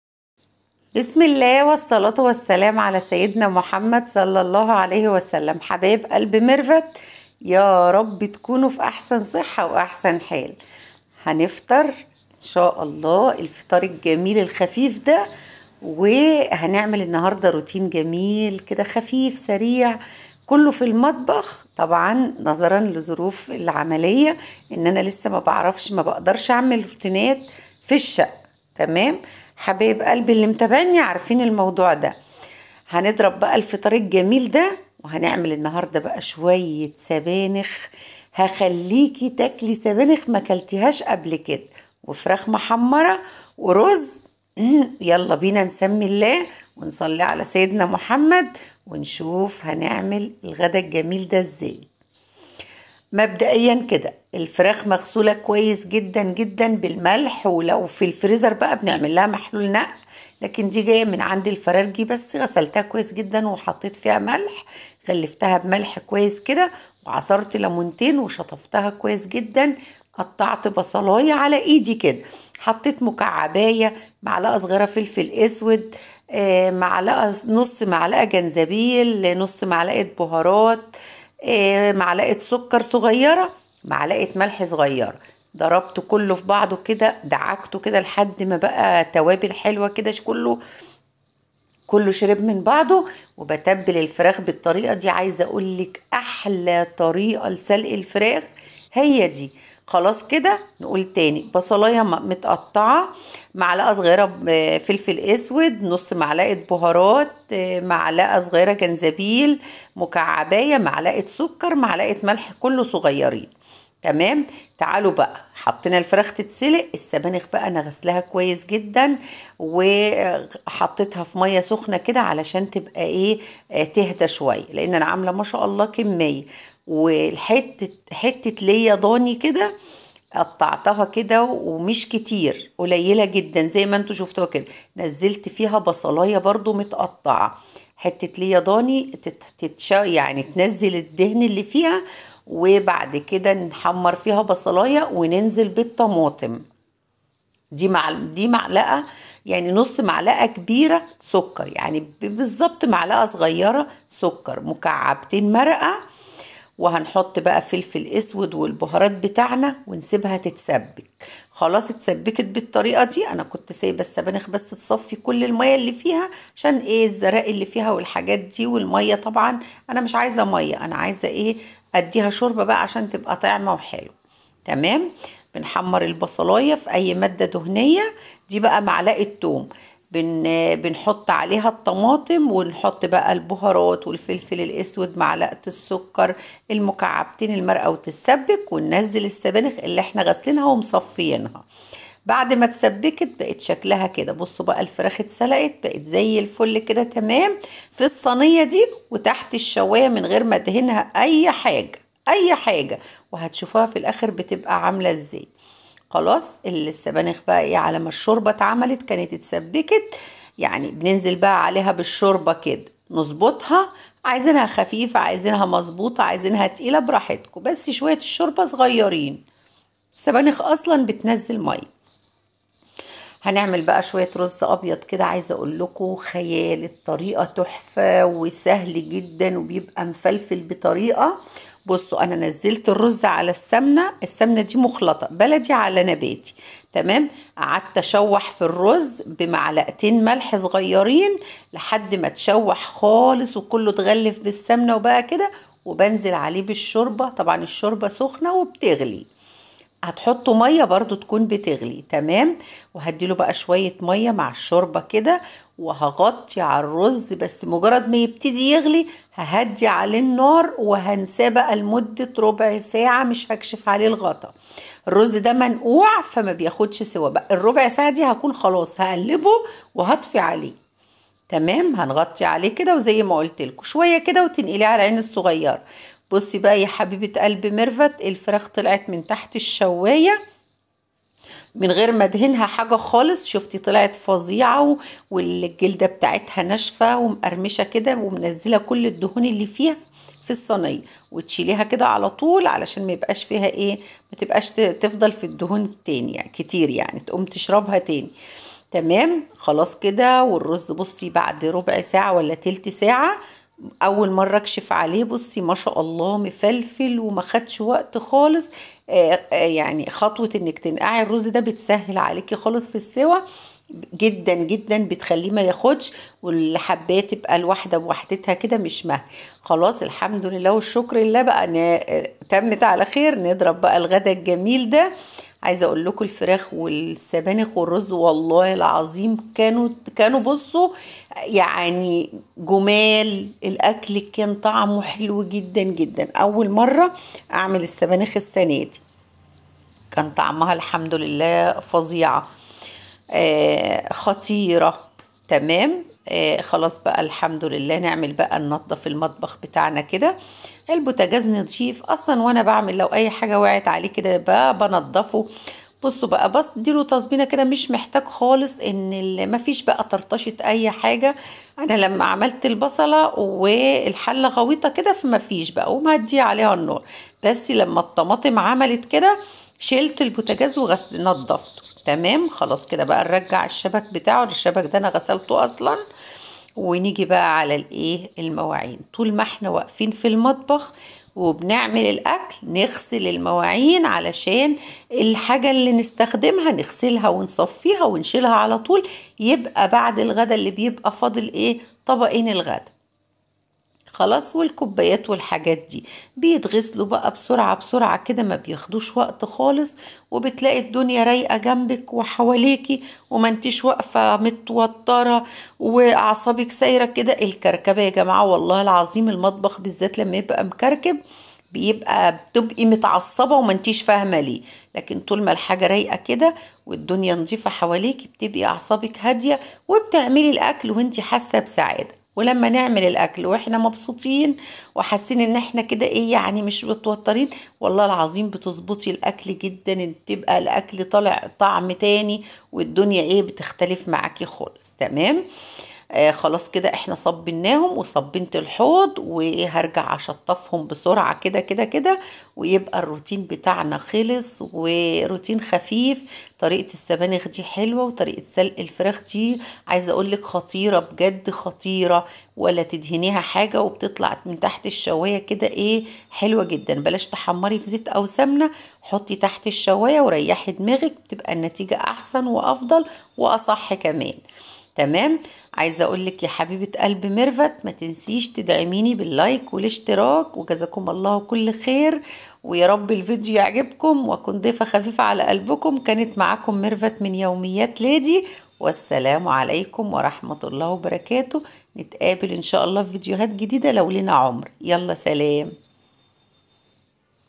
روتين الست المصرية إللي مراعيه بيتها ولا سلفه تشمت فيها ولا حماتها تقطم فيها وجوزها راضي عليها😠💪
طريقة عمل السبانخ طريقة عمل الارز الابيض المصري طريقة عمل الفراخ روتينات منزليه في المطبخ تنظيف المطبخ تنظيف الشقه